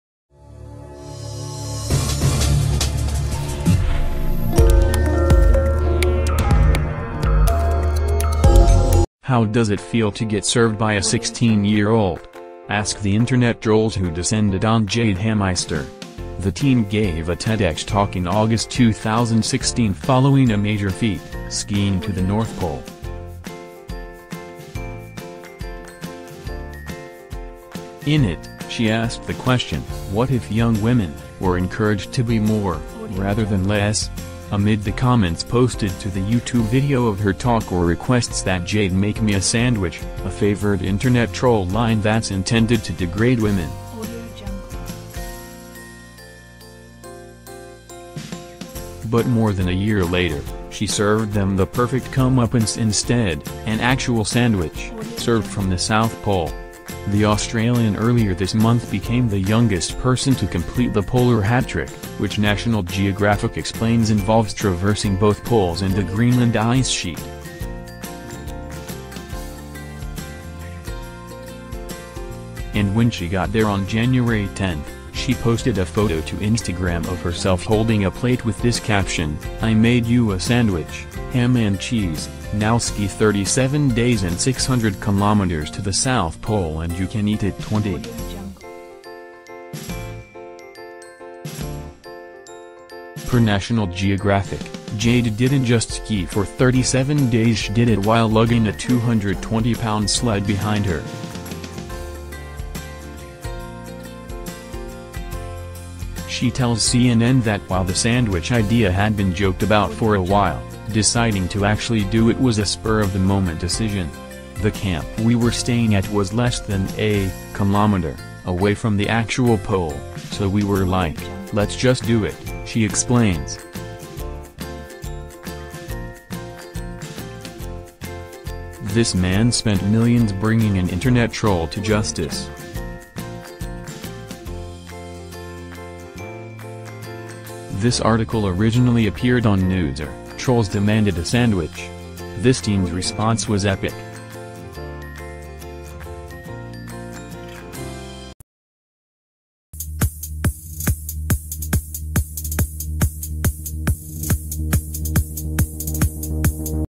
Internet trolls demanded a sandwich. The teen's response was epic
How does it feel to get served by a 16-year-old? Ask the internet trolls who descended on Jade Hameister. The teen gave a TEDx talk in August 2016 following a major feat: skiing to the North Pole.
In it, she asked the question, "What if young women ... were encouraged to be more, rather than less?" Amid the comments posted to the YouTube video of her talk were requests that Jade "make me a sandwich," a favored internet-troll line that's intended to degrade women.
But more than a year later, she served them the perfect comeuppance instead: an actual sandwich, served from the South Pole. The Australian earlier this month became the youngest person to complete the Polar Hat Trick, which National Geographic explains involves traversing both poles and the Greenland Ice Sheet.
And when she got there on Jan. 10, she posted a photo to Instagram of herself holding a plate with this caption, "I made you a sandwich (ham & cheese), now ski 37 days and 600km to the South Pole and you can eat it xx." Per National Geographic, Jade didn't just ski for 37 days—she did it while lugging a 220-pound sled behind her.
She tells CNN that while the sandwich idea had been joked about for a while, deciding to actually do it was a spur-of-the-moment decision. "The camp we were staying at was less than a [kilometer] away from the actual pole, so we were like, 'Let's just do it,'" she explains.
(This man spent millions bringing an Internet troll to justice.)
This article originally appeared on Newser: Trolls Demanded a Sandwich. This Teen's Response Was Epic